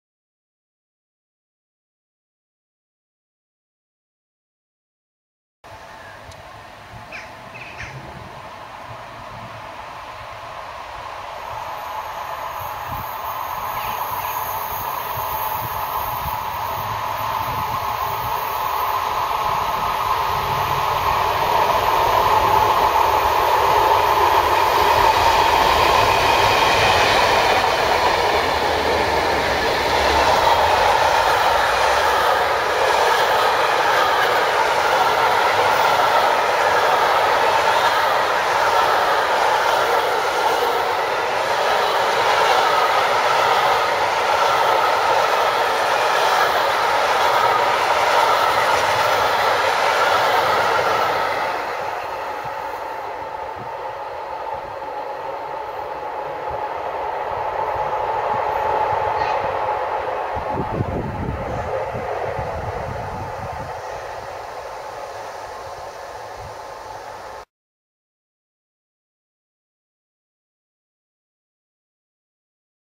CFL MX1023-1029-1807-1814 med cementtog
CFL MX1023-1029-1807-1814 med cementtog passerer Kolding den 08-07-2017